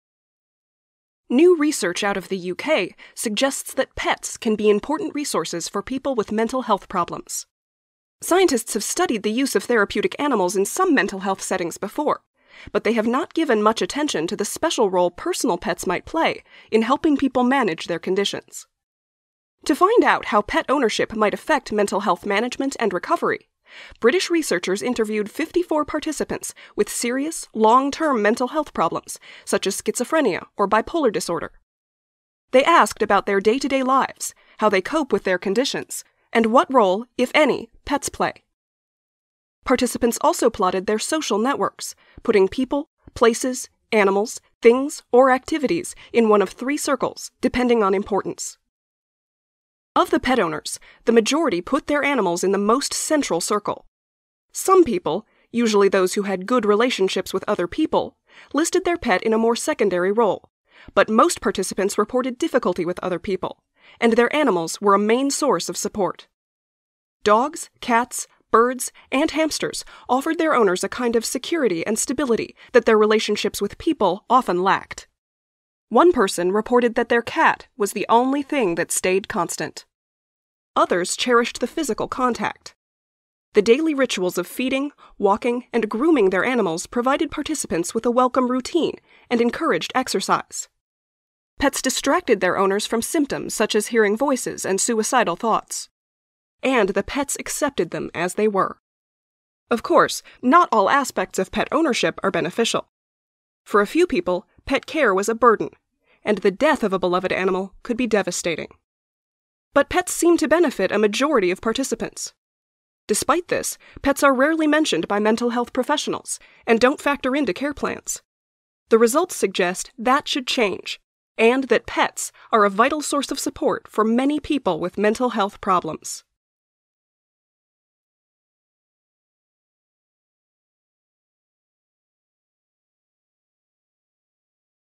The hidden work of pets in helping people with mental health problems
In a new video, we look at research recently published in BMC Psychiatry that explores the role of pets within the personal communities of those living with a mental health condition.